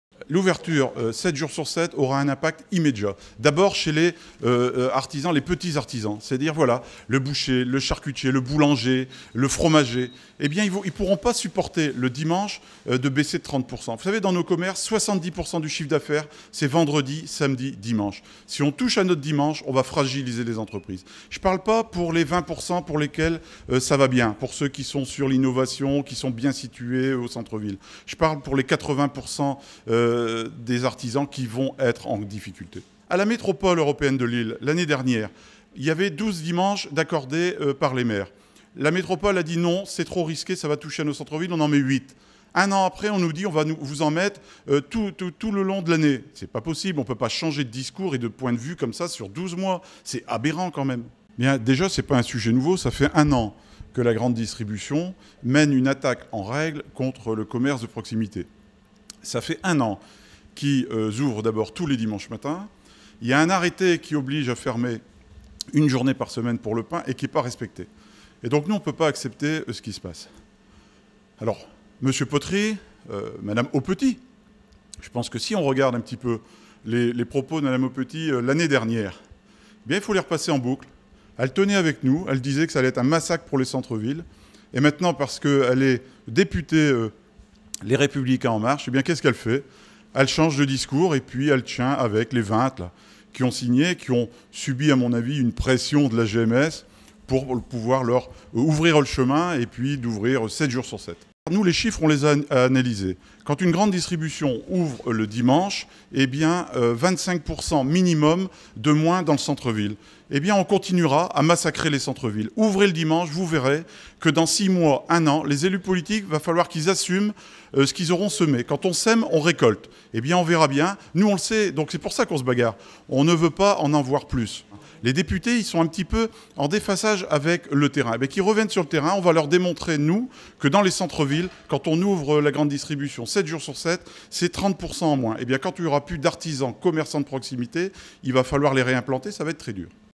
Ouverture Magasins le dimanche, un risque pour les petits commerçants ?
Le week-end du 18-19 août dernier, une vingtaine de députés LREM ont co-signés une tribune publiée dans le JDD. Celle-ci a pour but « d'aller plus loin » dans l’ouverture des magasins le dimanche. ⬇️
Notamment Benoit Potterie, député de la 8ème circonscription du Pas-de-Calais. Ces députés souhaitent mettre en pratique la "Loi pacte 2018" qui a pour objectif d'améliorer les performances des TPE et PME ainsi que d'associer les salariés aux résultats.
« Depuis de nombreuses années le chiffre des commerces baissent, et il faut réagir » expliquait le député sur le plateau de Grand Lille TV, notamment face à la concurrence du commerce sur internet. Ce n’est qu’une proposition, un débat.

L’U2P, L’Union des Entreprises de proximité, est quant à elle contre cette proposition, qui menacerait les petits commerces, comme les boucheries par exemple. 👇